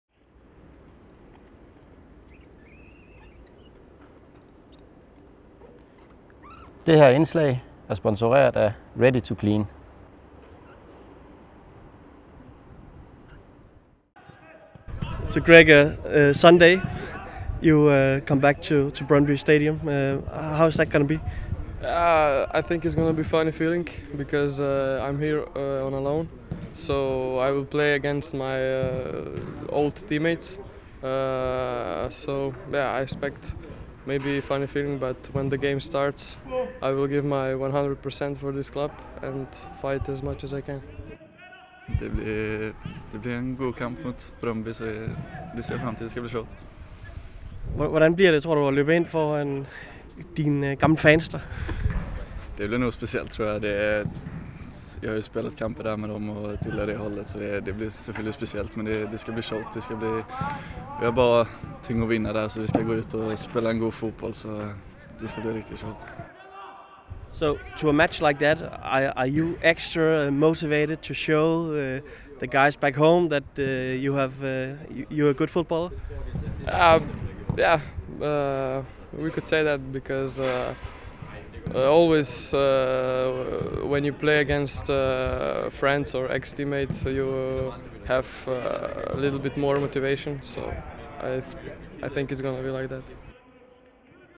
Sikošek: Er ekstra motiveret
Når Silkeborg IF søndag løber på banen mod Brøndby, så sker det med to lejesvende fra netop vestegnsklubben i Silkeborg-truppen. Gustaf Nilsson og Gregor Sikošek glæder sig begge til gensynet med Brøndby Stadion.